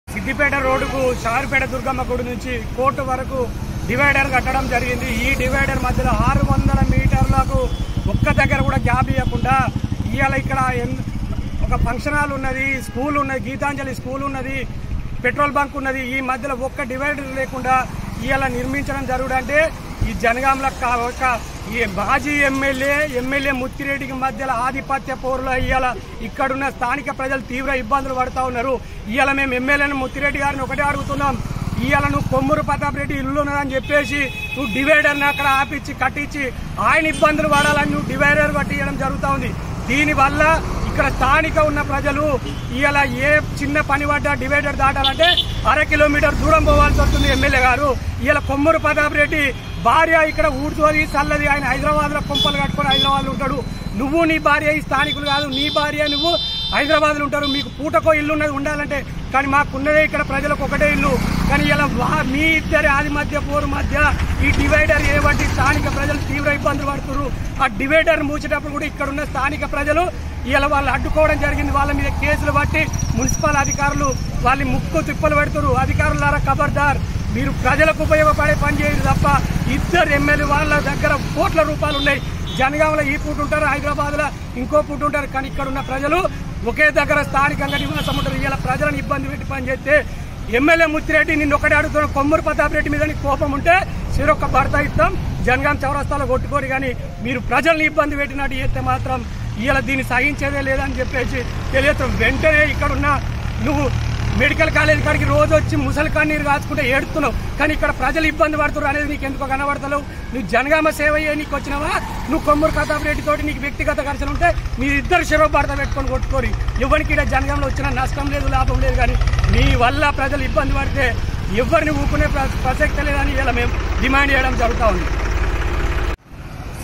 జనగామ ప్రజలను ఇబ్బంది పెట్టకండి ఎమ్మెల్యేముత్తిరెడ్డి || బెదిరిస్తే కేసుల.?బరిసలు ఇస్తాం కొట్టుకోండి
జనగామ ప్రజలను ఇబ్బంది పెట్టకండి ఎమ్మెల్యేముత్తిరెడ్డి || బెదిరిస్తే కేసుల.?బరిసలు ఇస్తాం కొట్టుకోండి - బీజేవైఎం మహిపల్